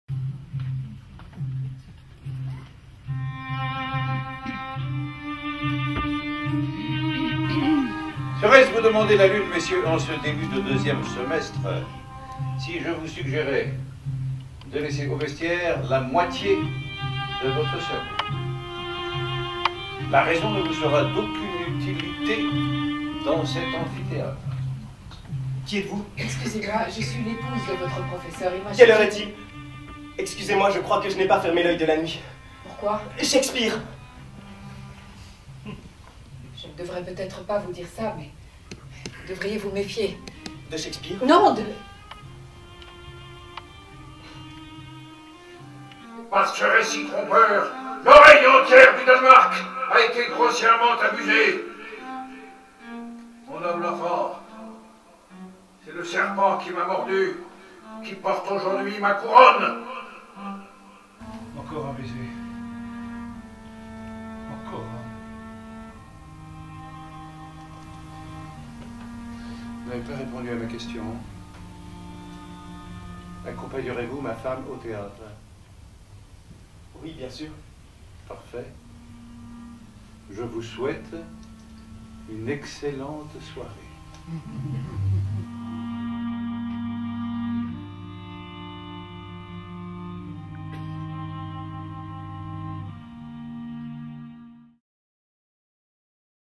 La confusion des sentiments
Un jeune étudiant, que les études universitaires rebutent et qui préfère courir les filles, rencontre un vieux professeur charismatique, qui lui donne le goût des études...et provoque entre eux une confusion des sentiments. Qu'est-ce qui se cache derrière cette amitié intellectuelle, derrière cet intérêt de ce vieux professeur pour son jeune élève ? Jusqu'où peut conduire une passion commune pour un travail intellectuel, qu'y a-t-il chez ce vieil homme et son étrange épouse que le jeune homme ne voit pas ?